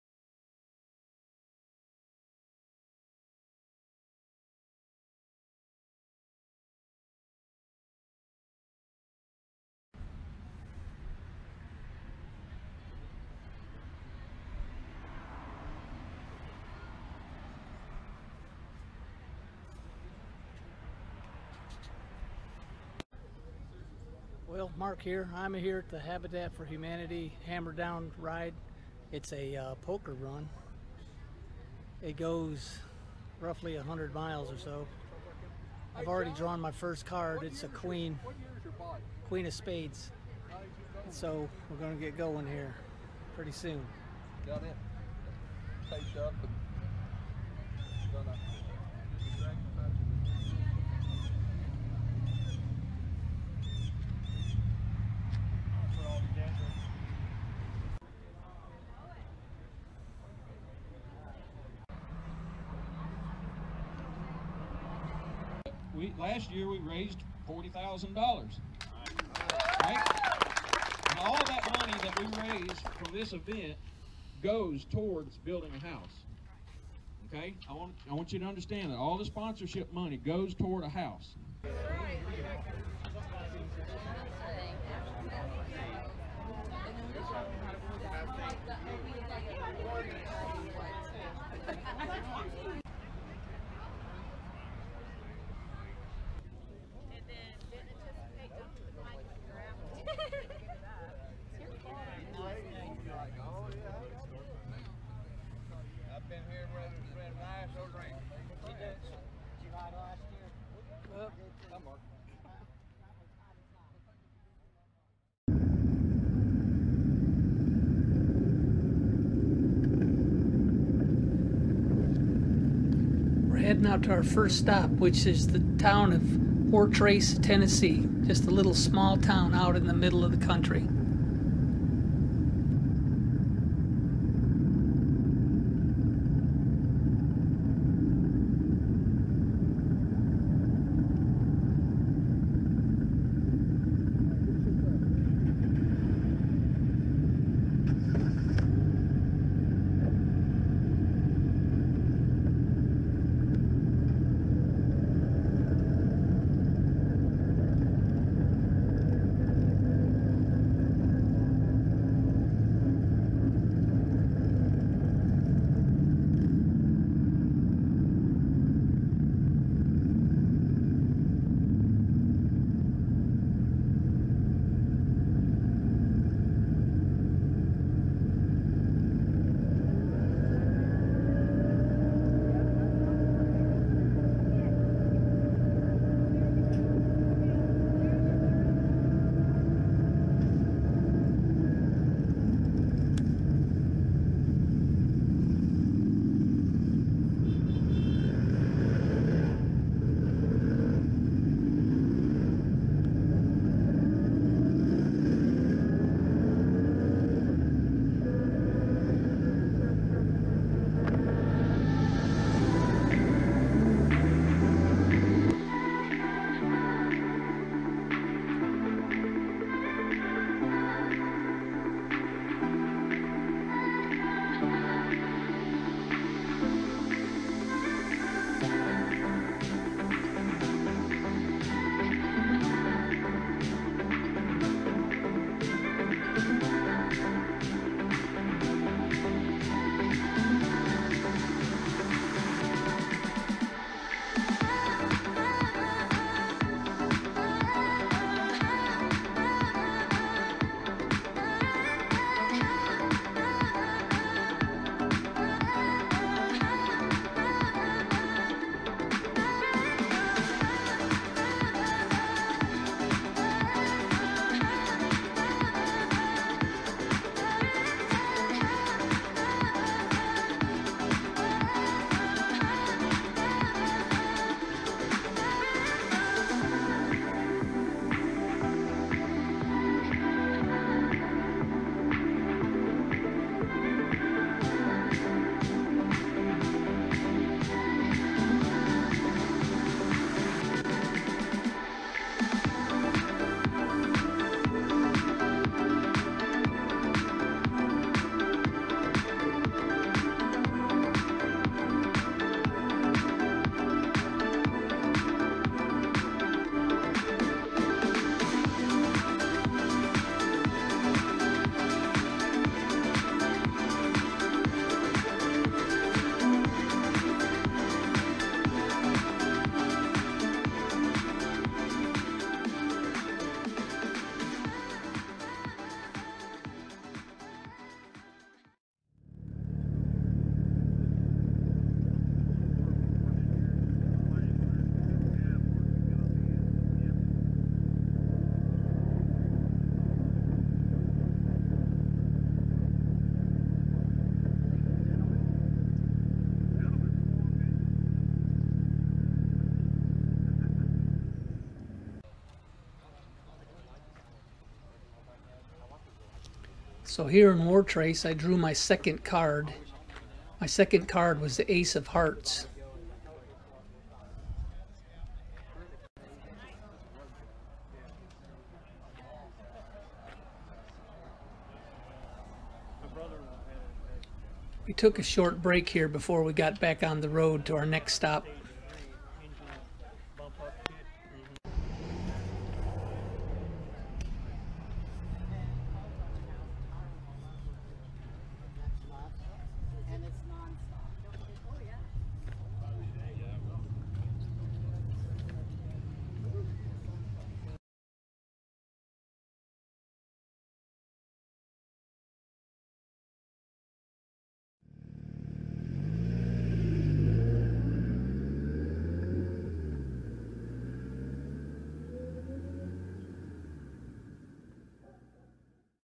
Habitat for Humanity ride part 1 sept 21 2024
I participate in a poker run to benefit Habitat for Humanity